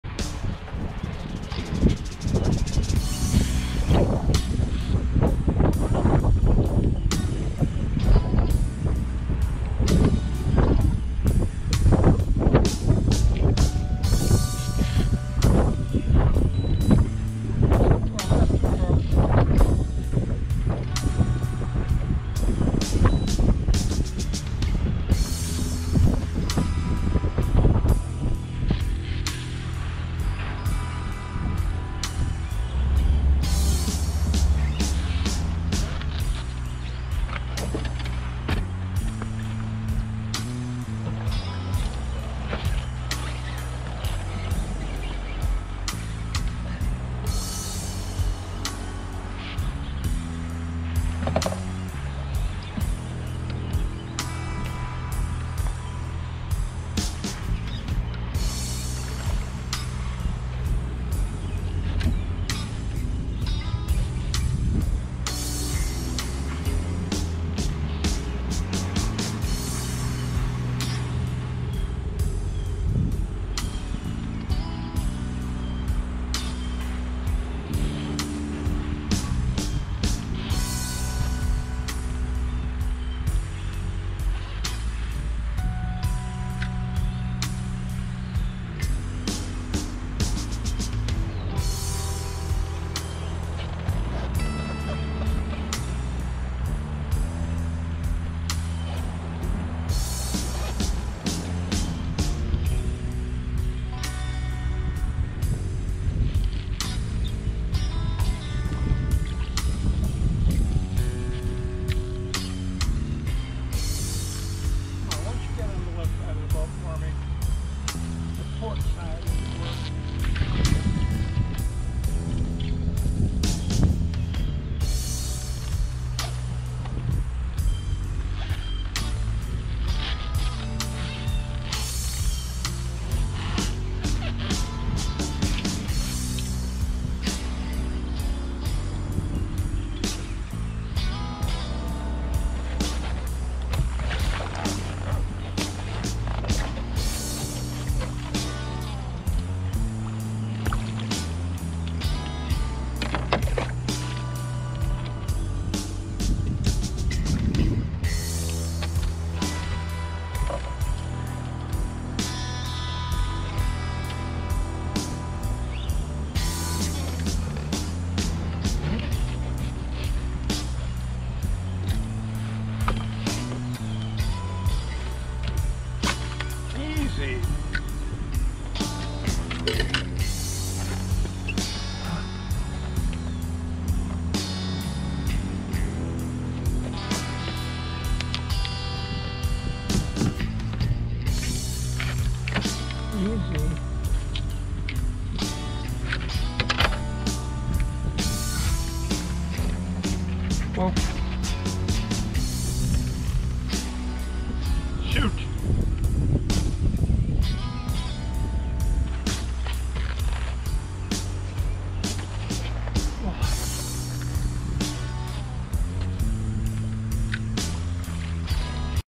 Time for a quick stop on the way back to the ramp?
Figured I could spare ten minutes or so to try to get even where I'd broken off my first fish of the day a couple hours earlier.